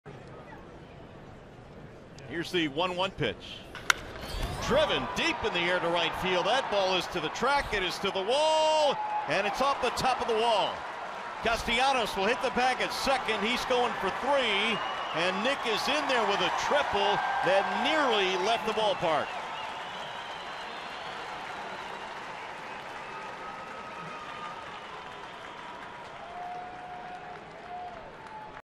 DET@TB: Castellanos triples off the right-field wall
Nicholas Castellanos drives a ball off the wall in right field, hustling around the bases for a triple in the top of the 1st inning